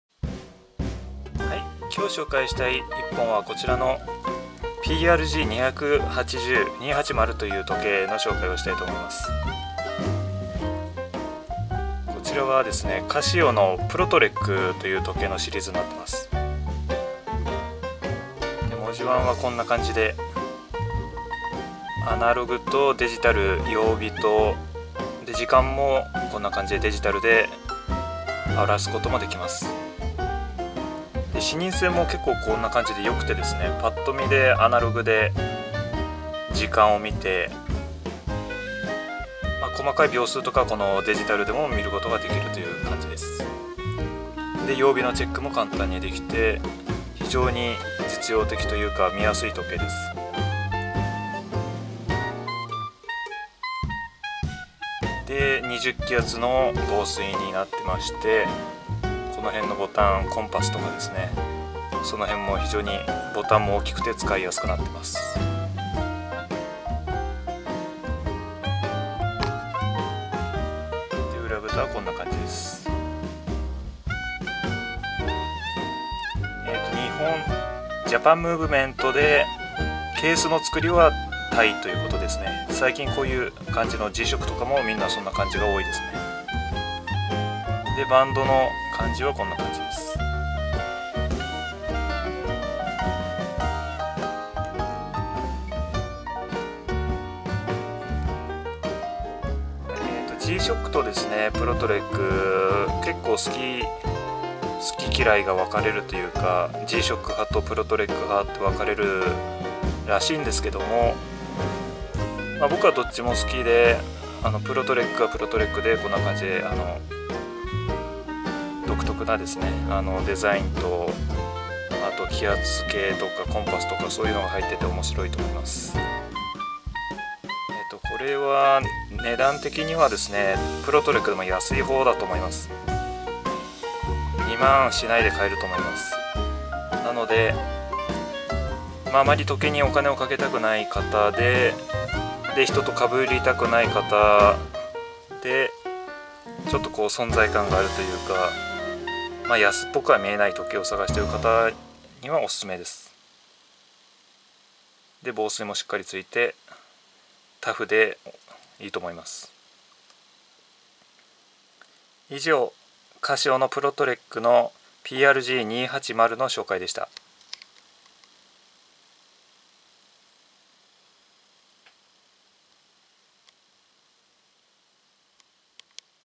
【時計レビュー】カシオのプロトレック(PRG280)時計紹介
アウトドアや登山、ハイキングにピッタリの時計です。